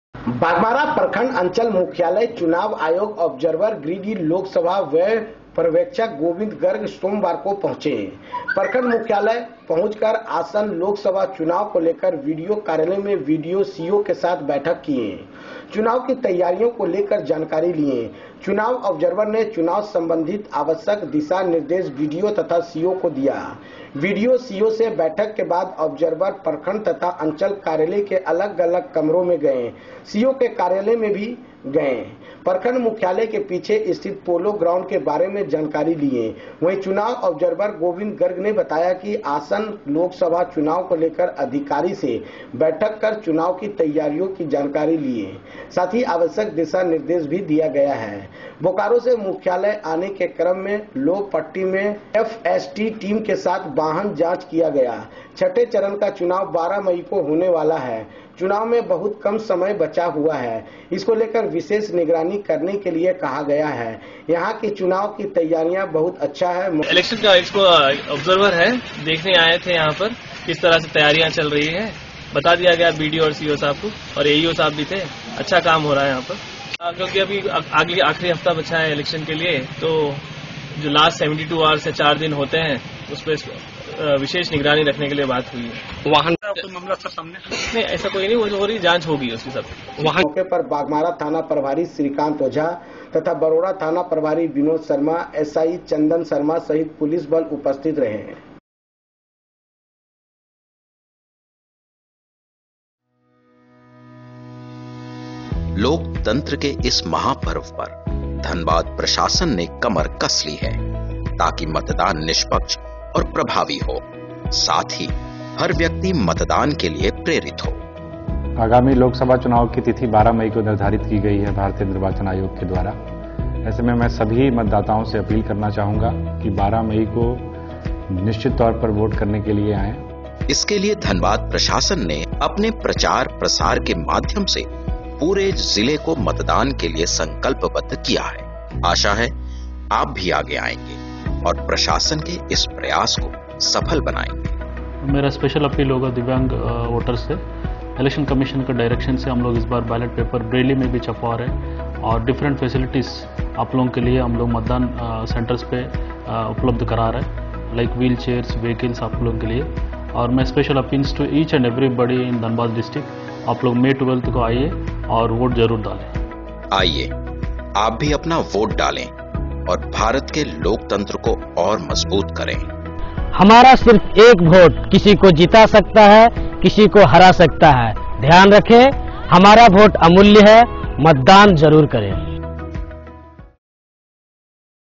Aaj ka sach news,,06.5.2019
सघन वाहन जांच के साथ रखे सभी पर नजर छठे चरण चुनाव की तिथि है नजदीक -- चुनाव ऑब्जर्वर
बाघमारा -- बाघमारा प्रखण्ड अंचल मुख्यालय चुनाव आयोग ऑब्जर्वर गिरिडीह लोकसभा व्यय प्रेक्षक गोविंद गर्ग सोमवार को पहुचे।प्रखण्ड मुख्यालय पहुच आसन लोकसभा चुनाव को लेकर बीडीओ कार्यालय में बीडीओ,सीओ के साथ बैठक किये।चुनाव तैयारी को लेकर जानकारी लिए।चुनाव ऑब्जर्वर ने चुनाव सम्बंधित आवश्यक दिशा निर्देश बीडीओ तथा सीओ को दिया।बीडीओ, सीओ से बैठक के बाद ऑब्जर्वर प्रखण्ड तथा अंचल कार्यालय के अलग अलग कमरों में गए।सीओ के कार्यालय में भी गए।प्रखण्ड मुख्यालय के पीछे स्थित पोलो ग्राउंड के बारे में जानकारी लिए।वही चुनाव ऑब्जर्वर गोविंद गर्ग ने बताया कि आसन लोकसभा चुनाव को लेकर अधिकारी से बैठक कर चुनाव तैयारी की जानकारी लिए।साथ आवश्यक निर्देश दिया गया है।बोकारो से मुख्यालय आने के क्रम में लोहपिट्टी में एफएसटी टीम के साथ वाहन जांच किया गया।छठे चरण का चुनाव 12 मई को होने वाला है।चुनाव में बहुत कम समय बचा हुआ है।इसको लेकर विशेष निगरानी करने के लिये कहा गया है।यहां की चुनाव तैयारी बहुत अच्छा है।मौके पर बाघमारा थाना प्रभारी श्रीकांत ओझा तथा बरोरा थाना प्रभारी विनोद शर्मा,एसएसआई चंदन शर्मा सहित पुलिस बल उपस्थित रहे।असीसीटेंट जिला व्यय प्रेक्षक अजय पन्ना भी उपस्थित रहे।